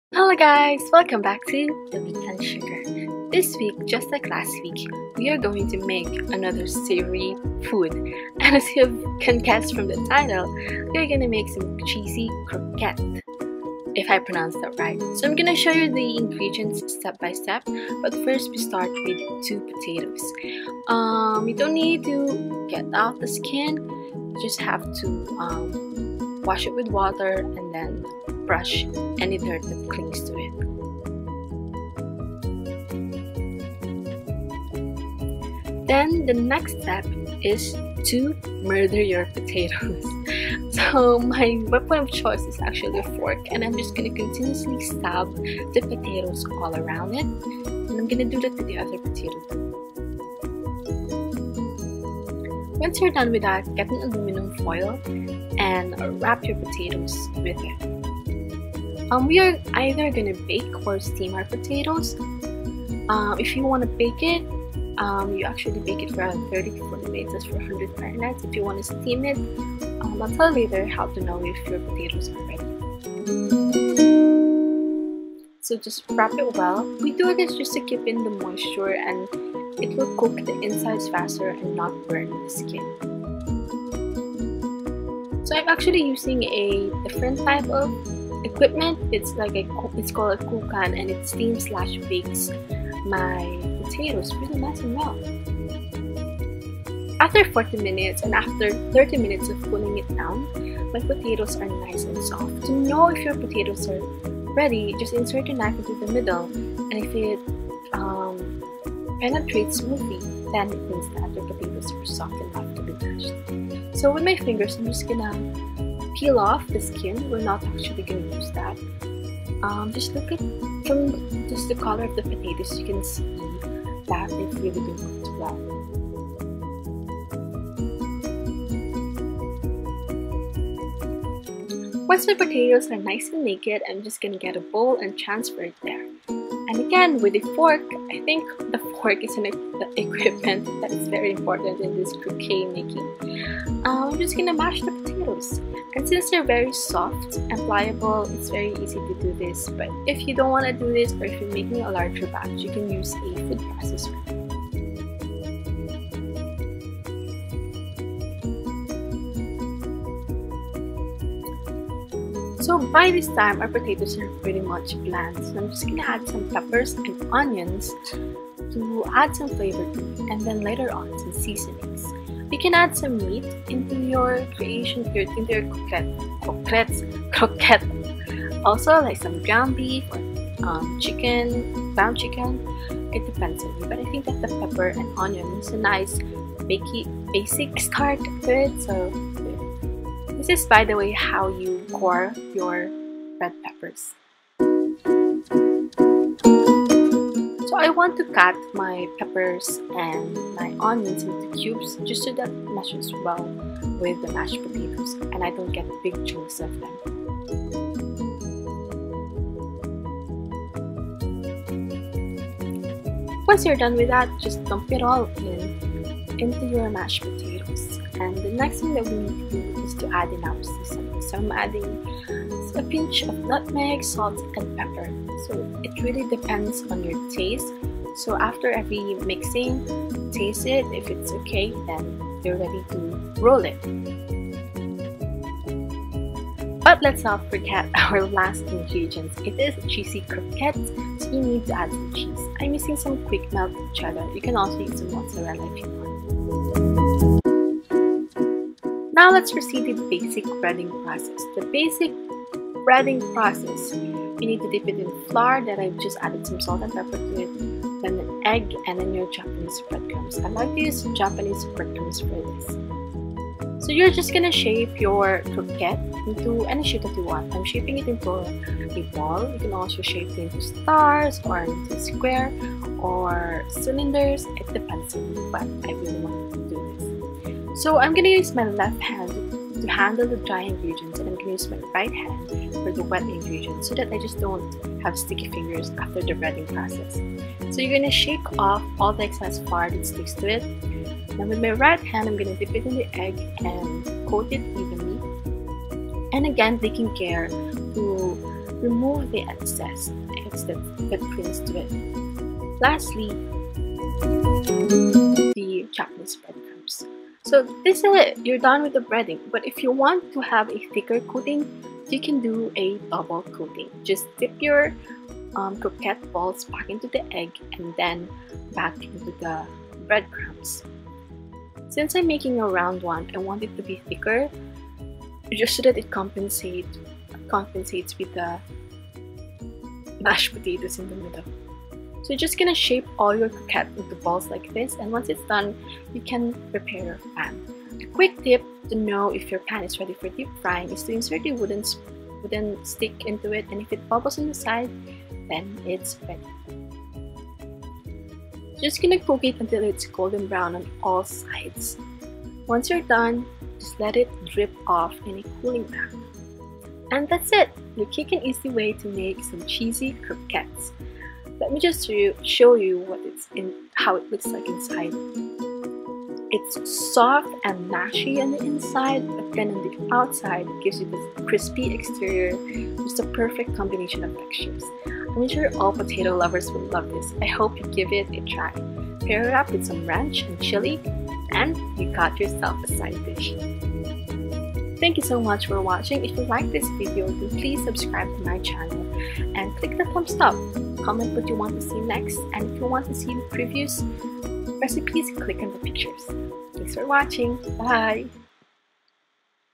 Cheesy Croquettes | The Weekend Sugar
The Weekend Sugar teaches you something savoury this week. Try out this cheesy croquette recipe! Perfect for potato lovers. Watch in HD!